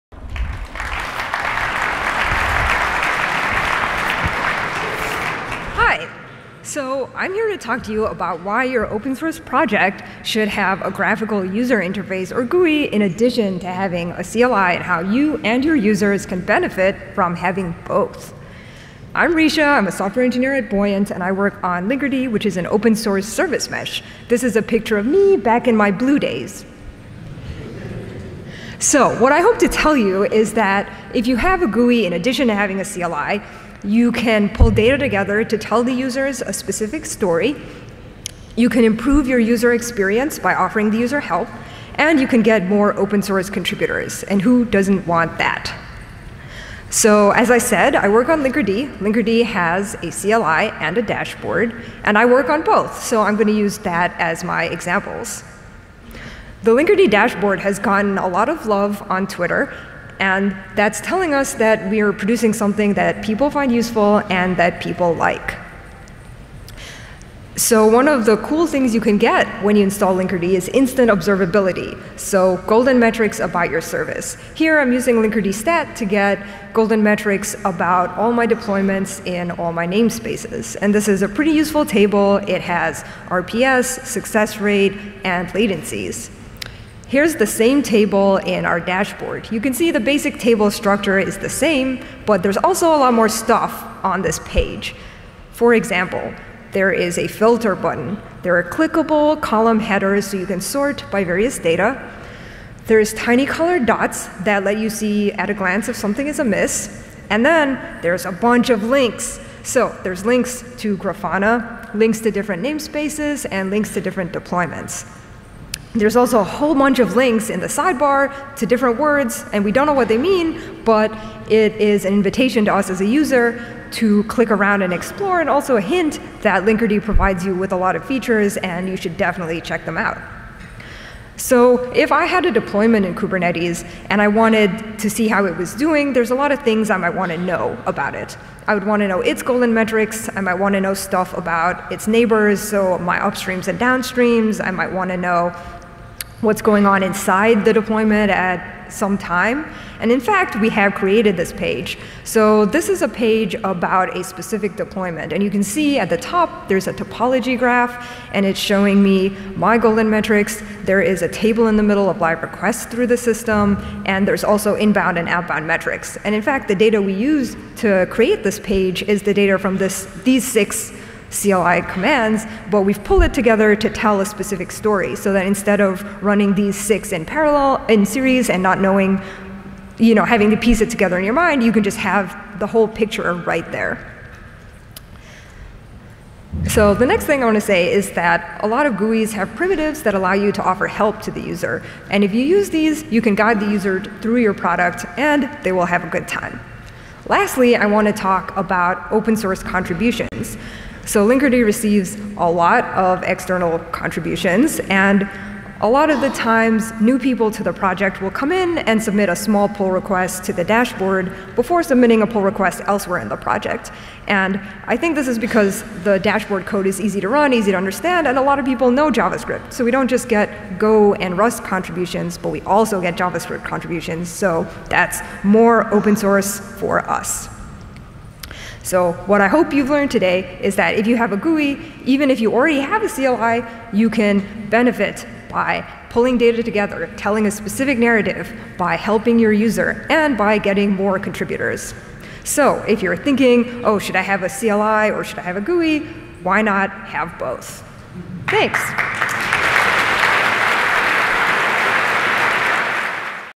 Lightning Talk: When the Command Line is Not Enough: Why Your OSS Project Needs A GUI - Risha Mars,
Lightning Talk: When the Command Line is Not Enough: Why Your OSS Project Needs A GUI - Risha Mars, Buoyant

In the ops and infrastructure world, we often rely on command line interfaces to do most of our heavy lifting. What value can a proxy get out of having a dashboard? How can a GUI complement a CLI? In this talk we'll look at Linkerd's web dashboard and CLI, and examine ways in which a GUI improves the user experience. In particular we’ll explore ways that a GUI offers more flexibility or better presentation capabilities than a CLI, namely: better data aggregation, easier data entry, better data manipulation, easier annotation and explanations with tooltips and popups, and user customization. In this talk, we’ll spend time looking at how different GUI elements (tables, charts, graphs, diagrams, tooltips, colour) can help the user get the most value of of a product. Lastly, we will talk about how GUIs can increase OSS participation by letting front-end coders contribute.